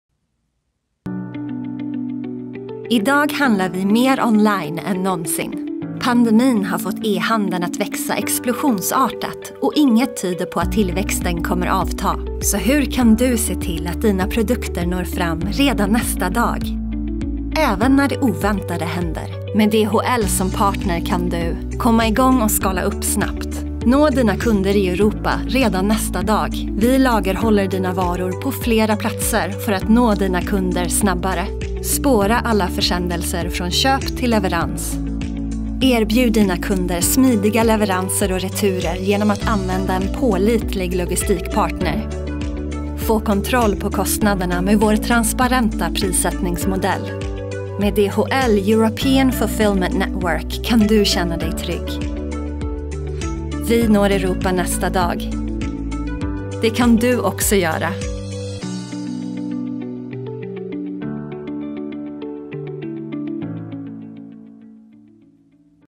DHL Fulfillment Network Sweden
Är du en liten eller mellanstor e-handlare? Med DHL European Fulfillment Network får du tillgång till vårt nätverk av 30 fulfillment-center. Vi möjliggör snabbare leverans. Allt för dina kunders skull